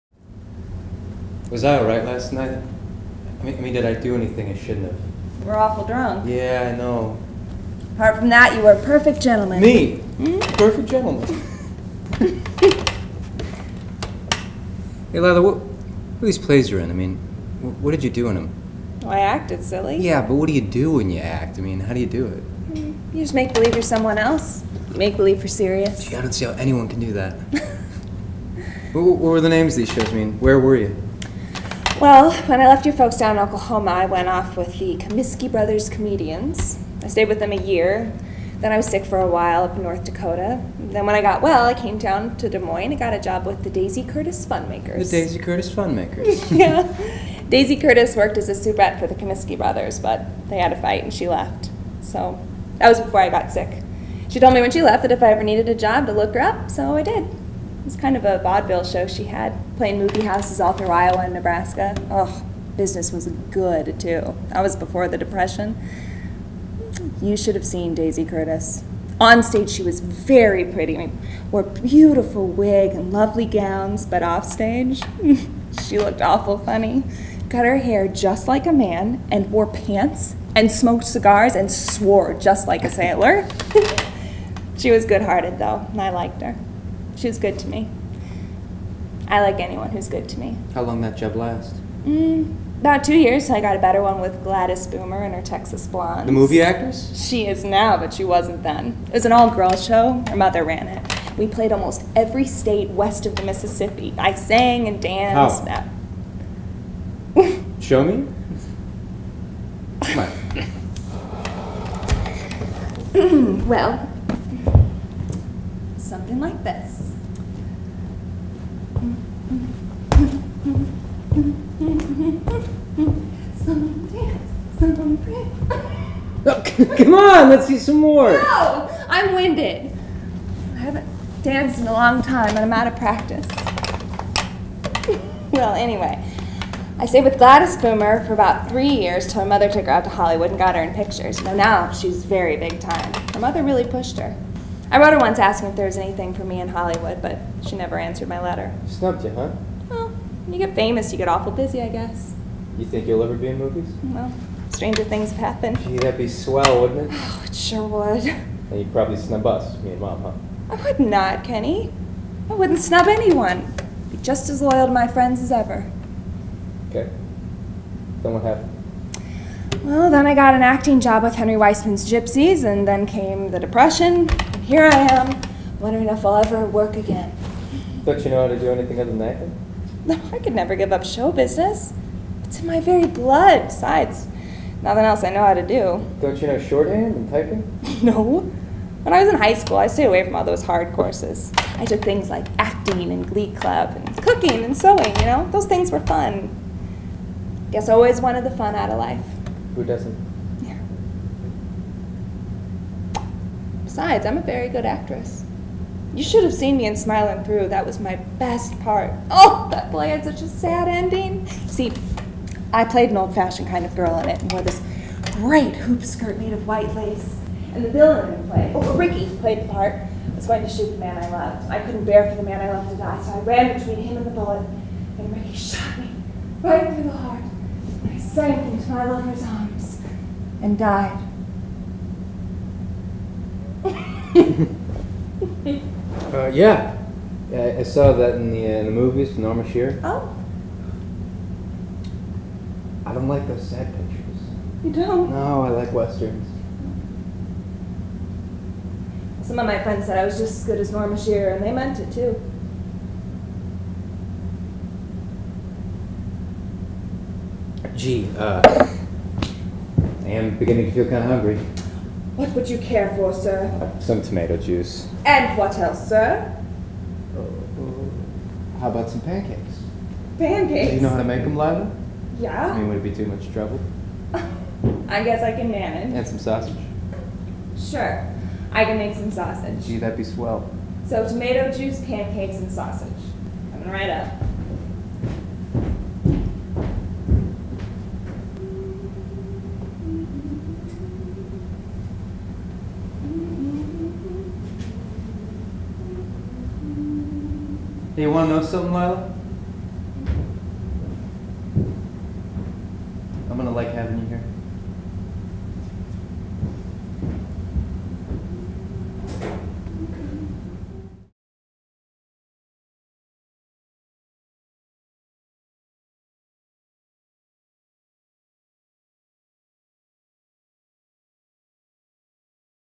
William Inge's "A Loss Of Roses" - Raven Theatre
A Loss of Roses is old-fashioned in nature, but rich in text and interpretation. Raven Theatre’s production, while a bit long, displays subtle strength and intellect at every turn. It’s no quick fix, but rather, a rewarding slow burn. Highly Recommended!